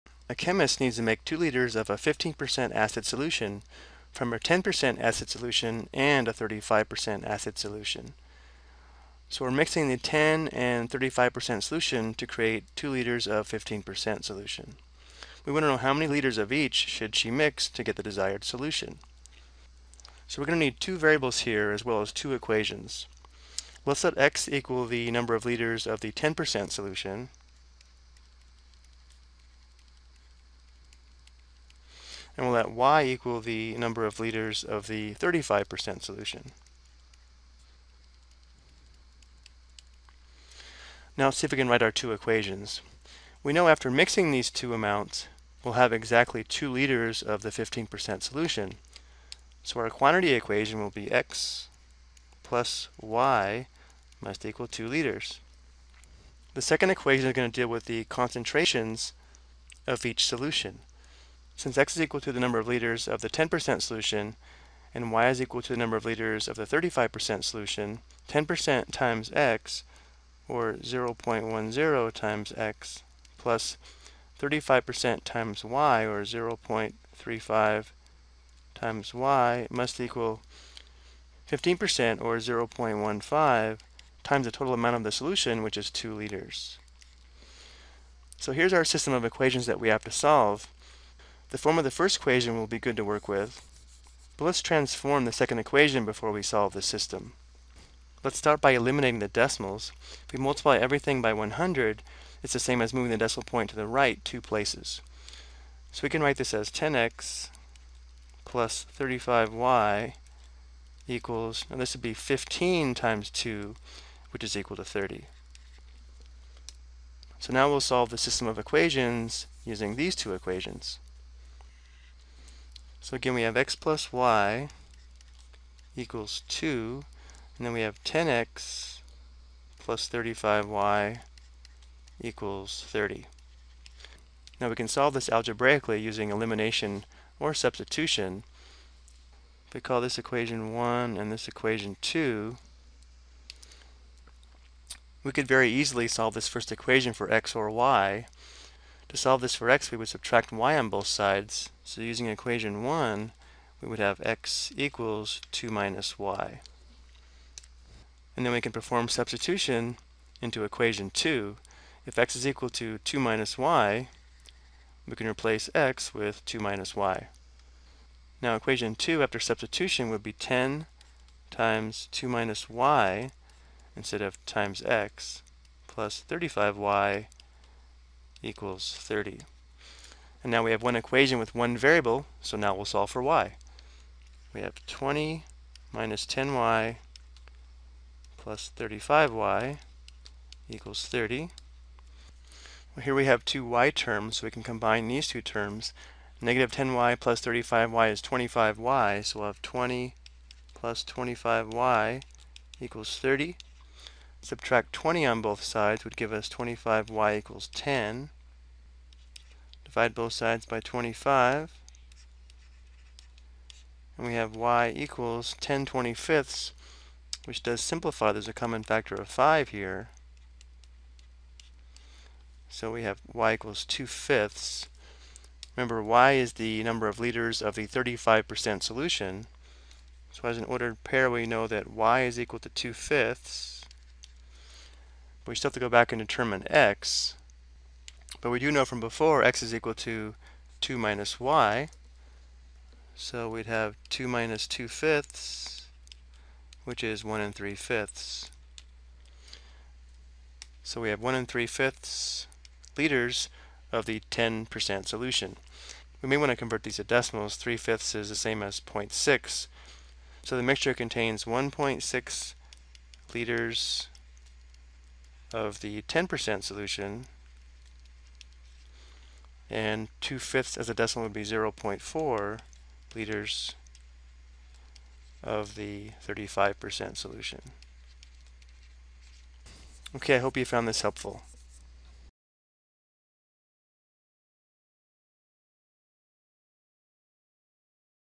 Ex: System of Equations Application - Mixture Problem
This video explains an application problem involving mixing two different concentrations of acid to make a different concentration.